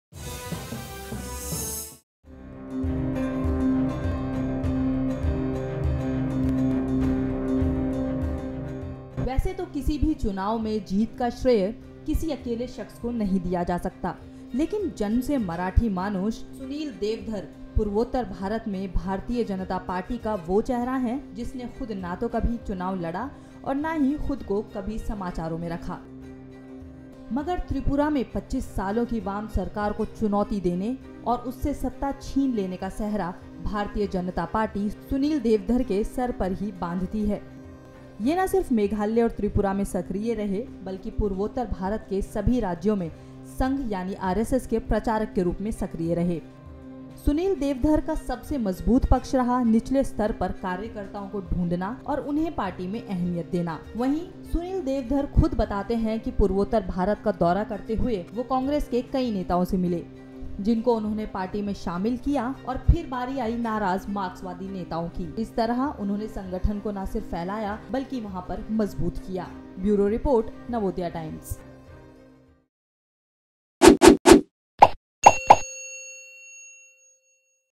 BJP को शानदार जीत दिलाने वाले Sunil Deodhar | Tripura Election 2018
चुनाव में जीत का श्रेय किसी अकेले शख्स को नहीं दिया जा सकता लेकिन भारतीय जनता पार्टी के जीत का चेहरा  है सुनील देवधर।